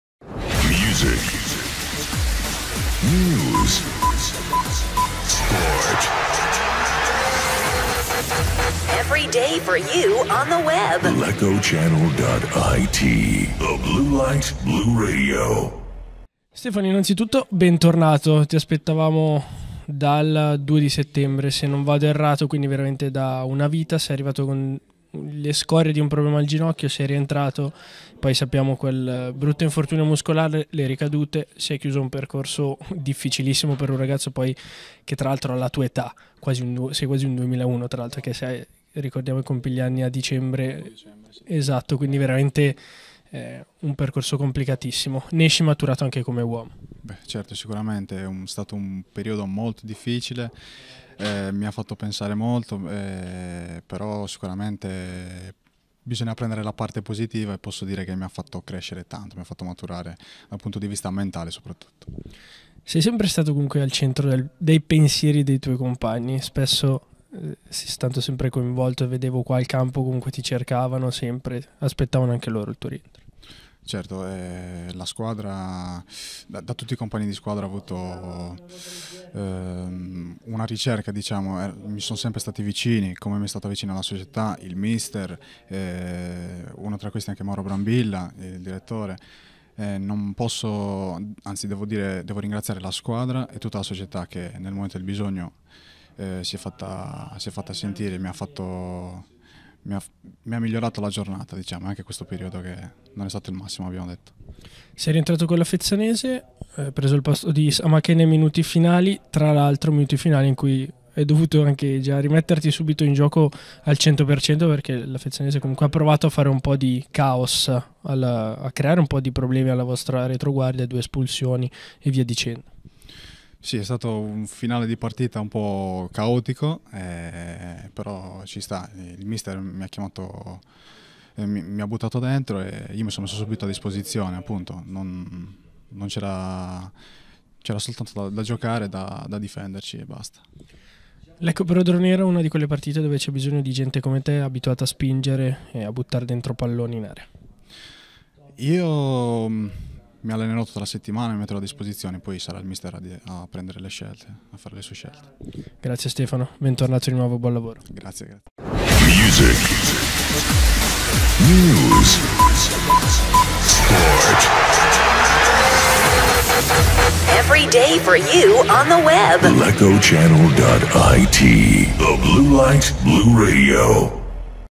Calcio Lecco, Ruiu: «Devo ringraziare tutti. Ho sensazioni positive»
Durante Fezzanese-Lecco il giovane terzino di scuola Cagliari ha fatto il suo esordio in campionato con la maglia bluceleste, al termine di un vero e proprio calvario.